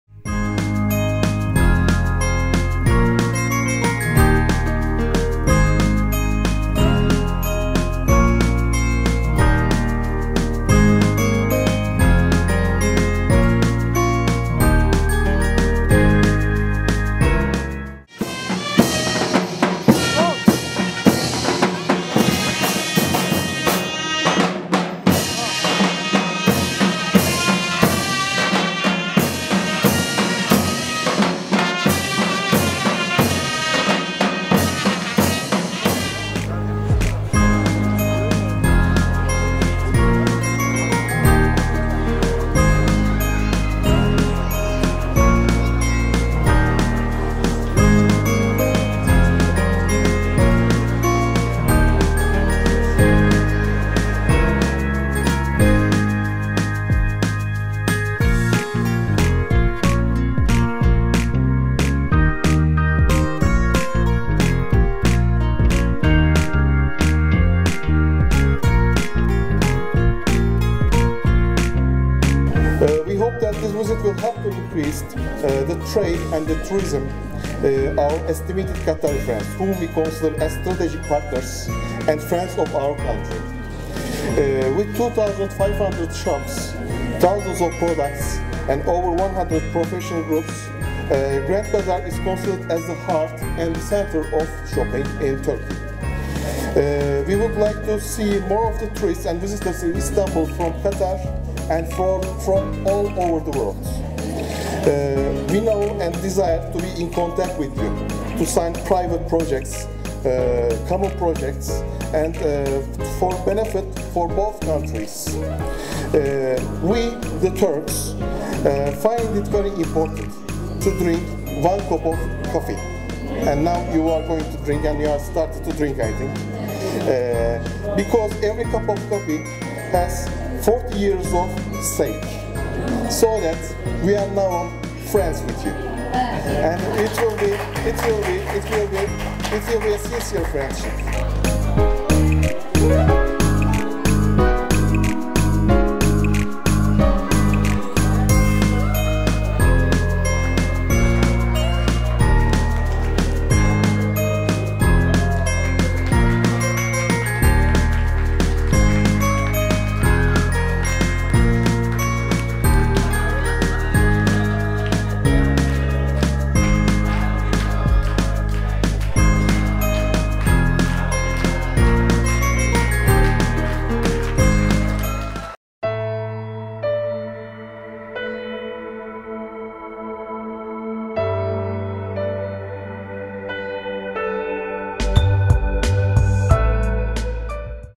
Katar'lı Turizm Acenatları Kapalıçarşı'da konuk edildi.
Kapalıçarşı Yönetim Kurulu Katarlı 13 Turizm Acentası sahibi ve müdürlerini çarşıda ağırladı...